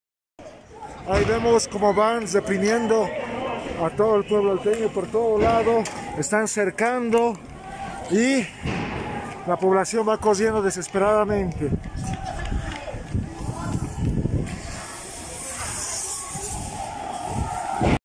Represión en contra el pueblo alteño
...ordenado por la flamante presidenta ilegítima de Bolivia